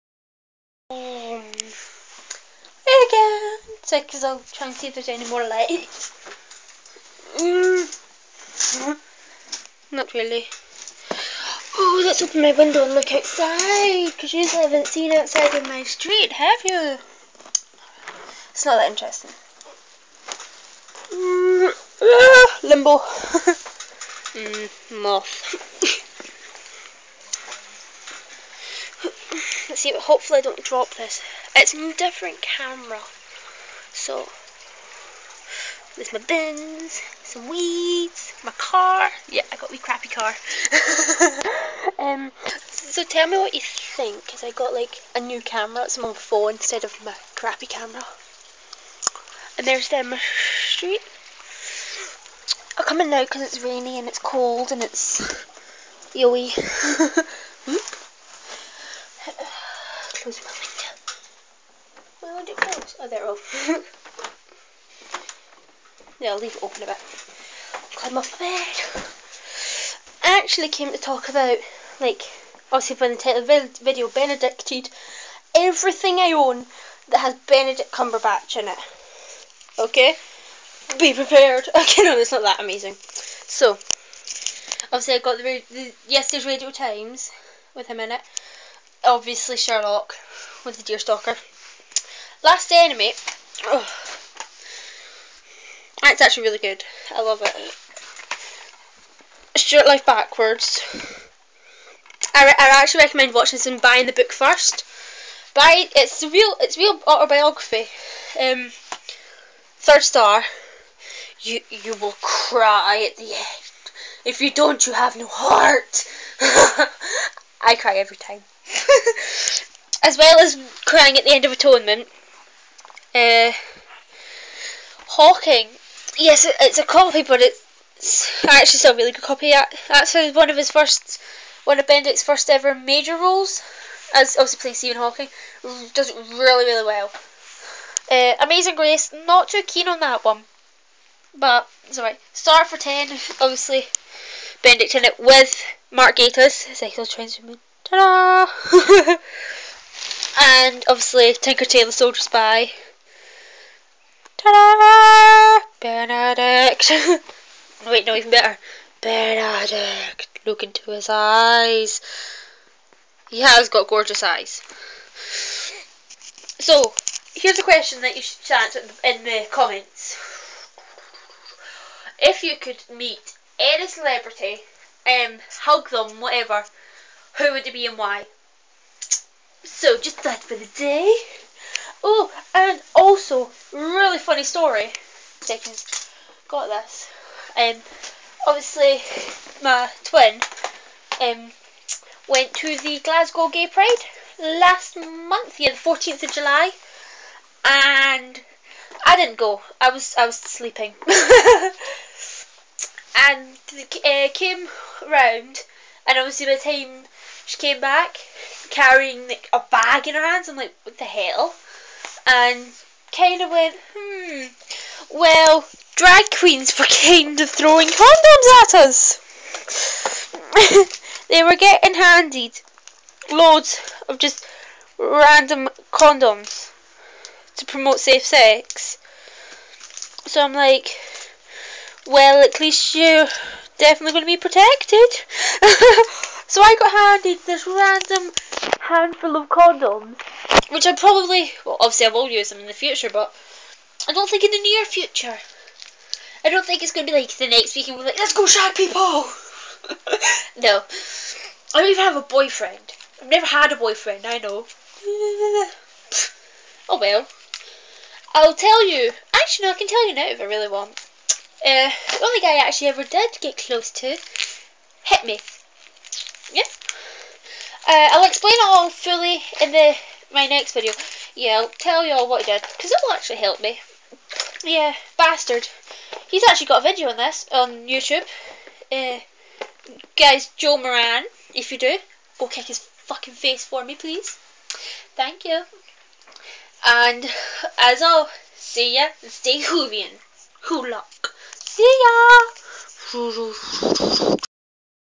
BENADDICTED (plus a mini rant)
I talk about all of my Benedict Cumberbatch films and a funny story involving drag queens and condoms.